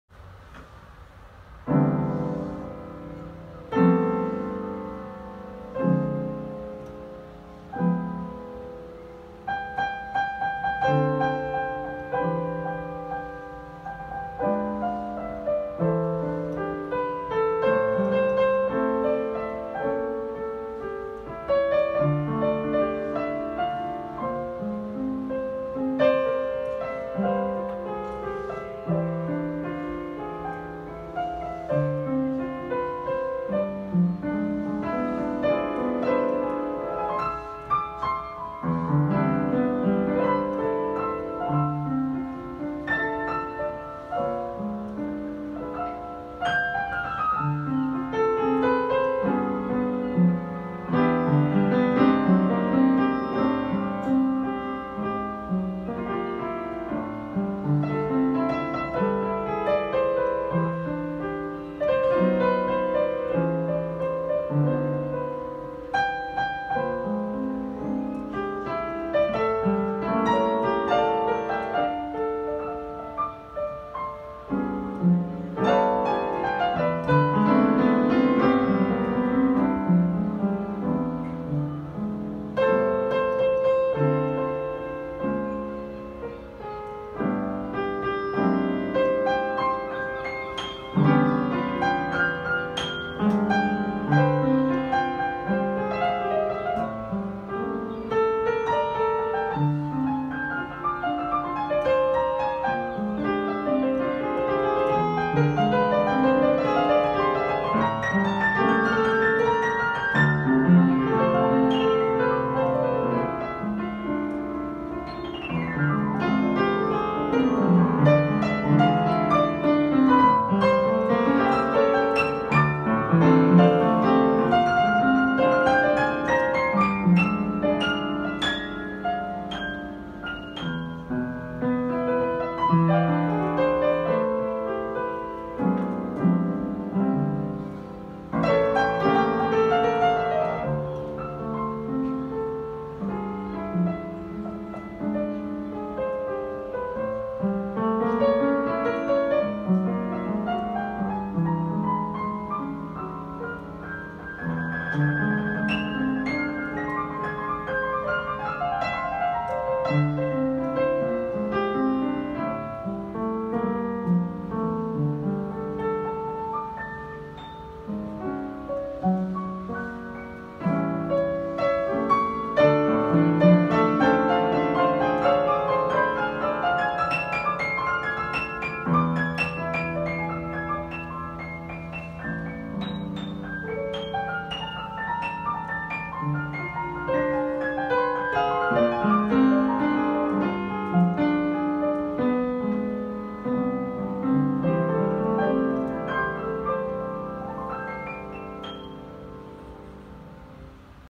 David Mannesse pianist
Romantic jazzy Improvisation 5 August 2017 at my Yamaha upright at the Dutch coast.